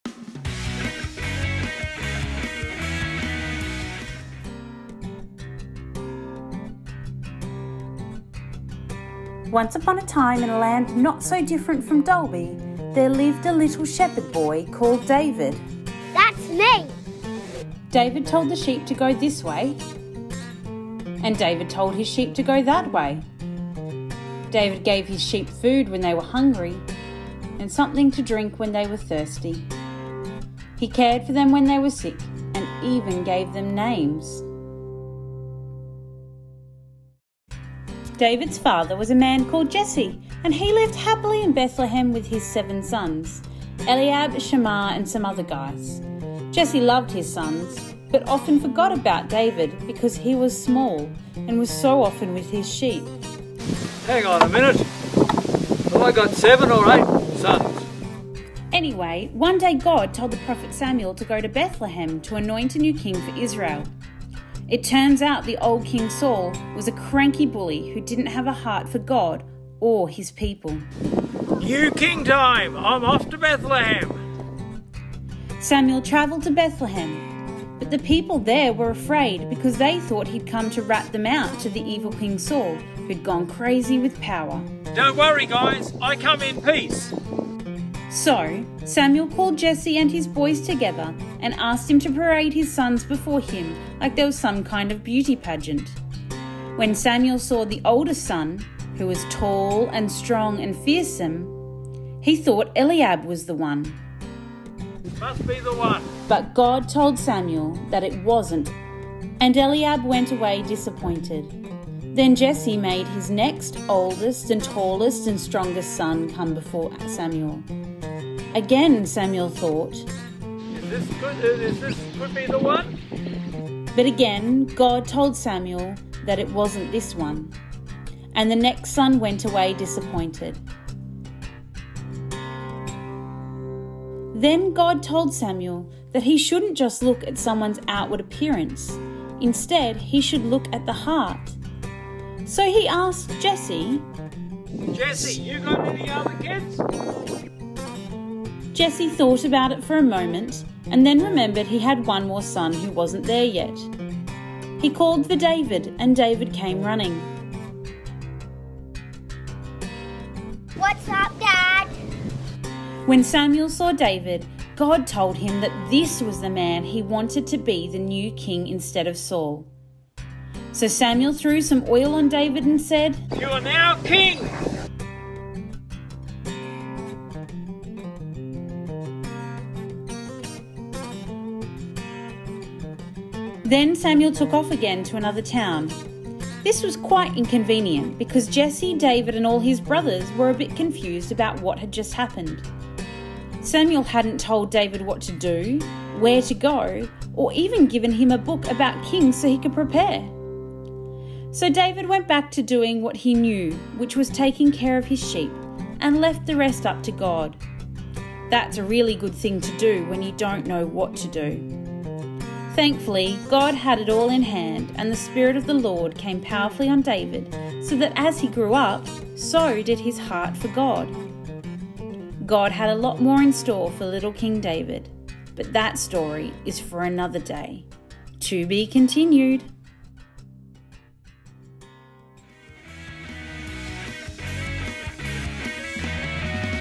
Kids Talk - A Matter of the Heart Week 1 (11 July 2021)
A Kids Talk based on 1 Samuel 16.